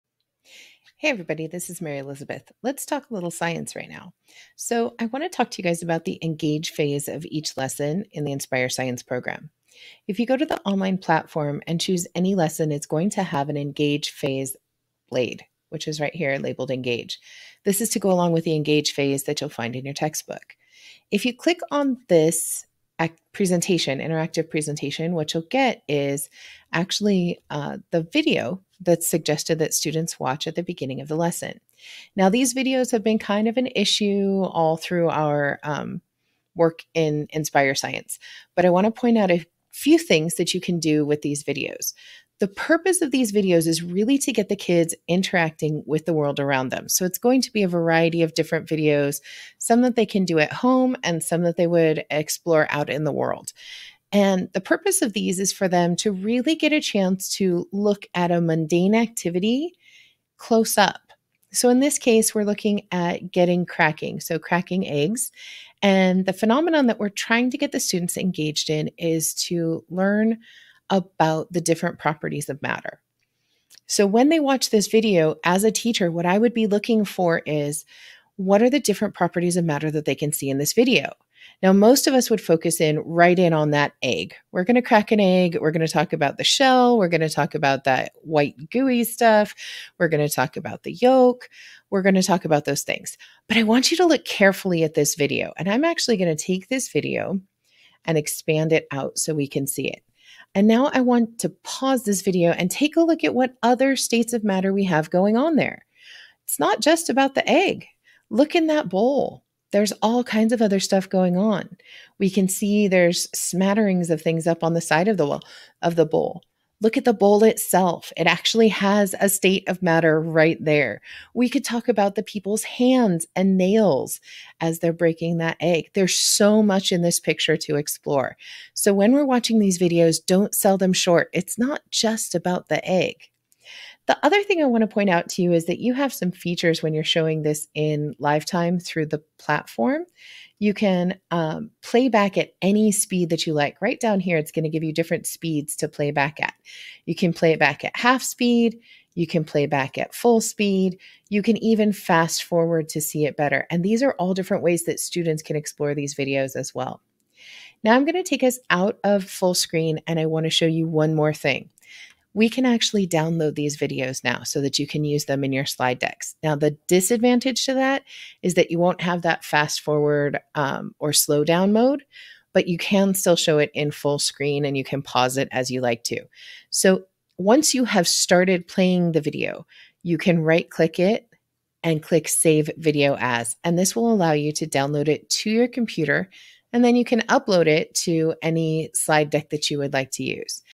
Inspire Science: Engage Phase Video Tips
In this video, teachers will learn how to use the videos for the engage phase of the Inspire Science program.